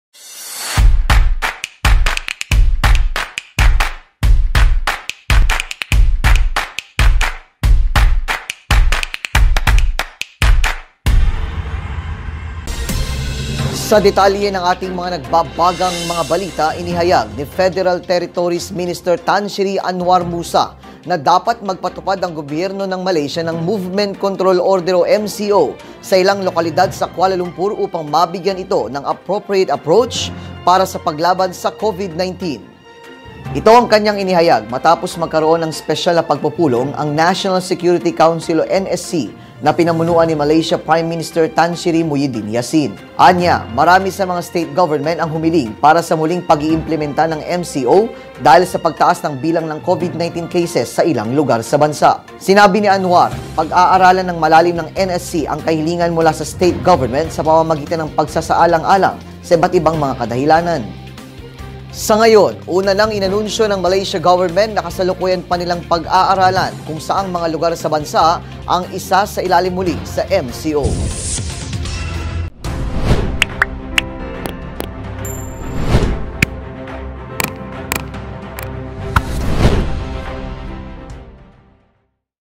Kuala Lumpur, dapat muling ilagay sa MCO ayon sa isang federal territory minister.
Inihayag ni Federal Territories Minister Tan Sri Annuar Musa na dapat magpatupad ang gobyerno ng Malaysia ng Movement Control Order o MCO sa ilang lokalidad sa Kuala lujmpur upang mabigyan ito ng appropriate approach para sa paglaban sa COVID-19.

Abangan pa ang maiinit na balita dito lang sa